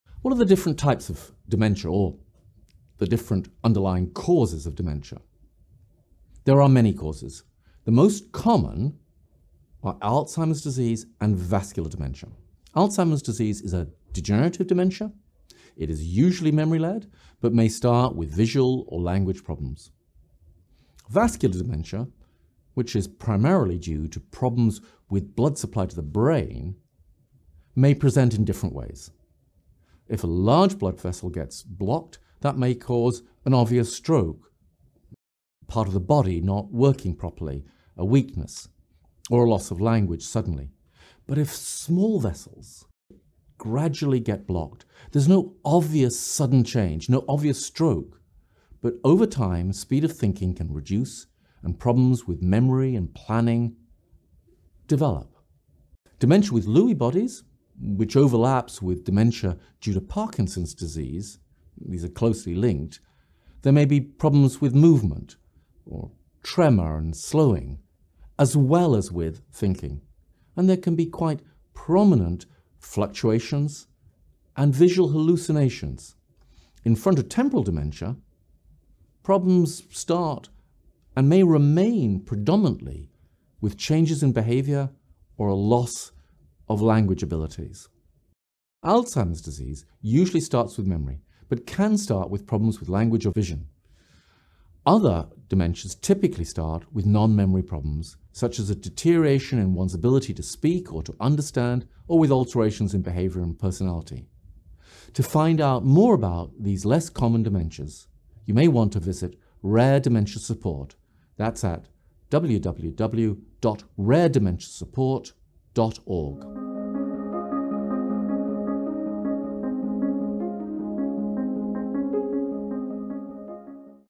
What causes dementia?
Consultant neurologist Professor Nick Fox talks us through what are the different types and underlying causes of dementia

Nick Fox is the professor of neurology and director of the Dementia Research Centre at the UCL Insitute of Neurology, Queen Square, London, and a consultant neurologist in the Cognitive Disorders Clinic. He co-leads Rare Dementia Support which provides support, help and advice on less common, young onset and inherited dementias.